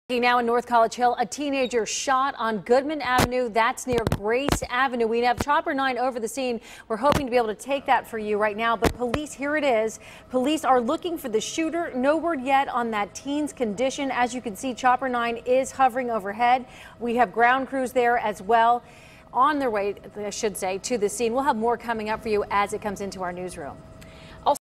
Teen with gunshot wound found in North College Hill
A teen with a gunshot wound was found Thursday afternoon in North College Hill.